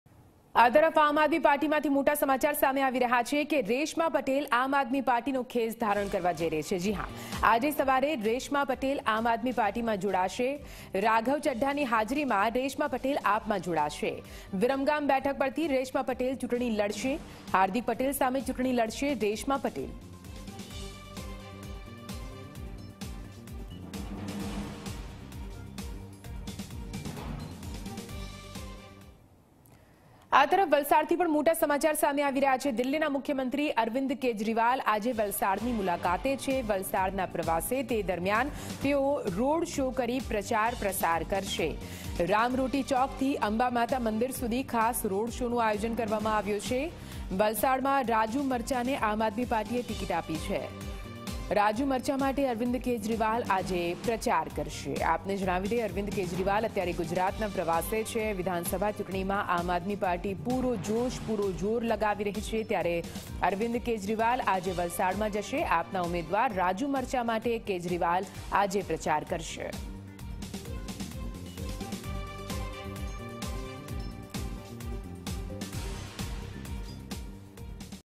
AAP Mission 2022:આમ આદમી પાર્ટીનો ખેસ ધારણ કરશે રેશમા પટેલ |હાર્દિક પટેલ સામે ચૂંટણી લડશે રેશમા પટેલ
આમ આદમી પાર્ટીનો ખેસ ધારણ કરશે રેશમા પટેલ --
આજે સવારે રેશમા પટેલ જોડાશે આપમાં --
રાઘવ ચઢ્ઢાની હાજરીમાં રેશમા જોડાશે આપમાં --
વિરમગામ બેઠક પરથી રેશમા પટેલ લડશે ચૂંટણી --
હાર્દિક પટેલ સામે ચૂંટણી લડશે રેશમા પટેલ --